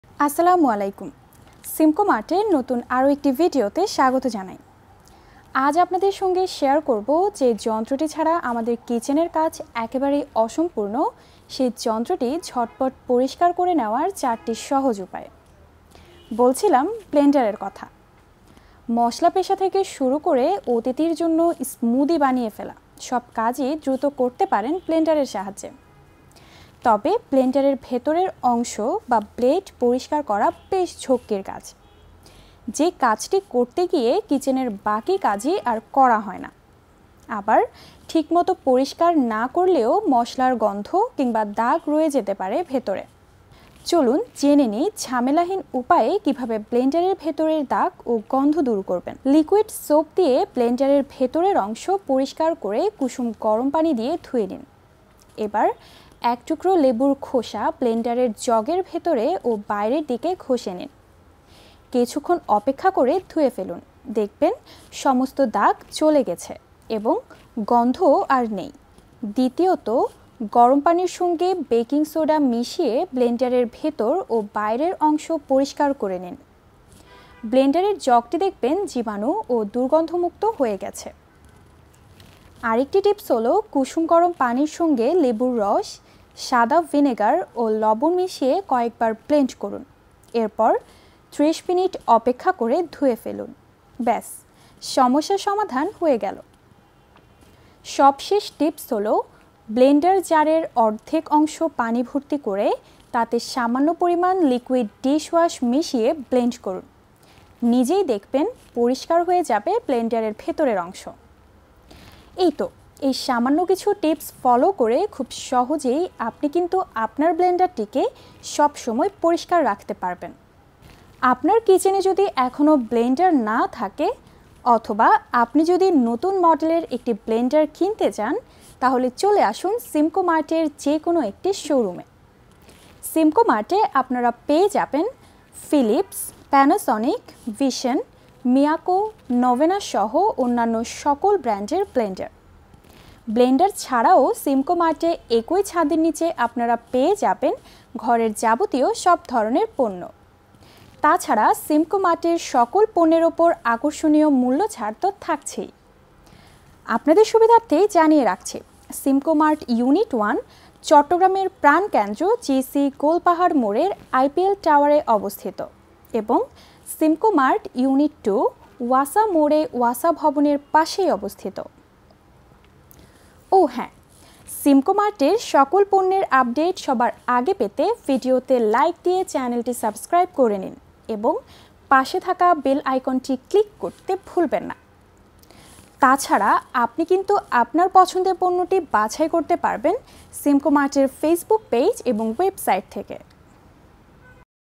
ব্লেন্ডার ঝটপট পরিষ্কার করার ৪ উপায় । । Simcomart
মসলা পেষা থেকে শুরু করে অতিথির জন্য স্মুদি বানিয়ে ফেলা- সব কাজই দ্রুত করতে পারেন ব্লেন্ডারের সাহায্যে। তবে ব্লেন্ডারের ভেতরের অংশ বা ব্লেড পরিষ্কার করা বেশ ঝক্কির কাজ। যে কাজটি করতে গিয়ে কিচেনের বাকি কাজে আর করা হয় না। আবার, ঠিক মতো পরিষ্কার না করলেও মসলার গন্ধ বা দাগ রয়ে যেতে পারে ভেতরে। চলুন জেনে নিই, ঝামেলাহীন উপায়ে কীভাবে ব্লেন্ডারের ভেতরের দাগ ও গন্ধ দূর করবেন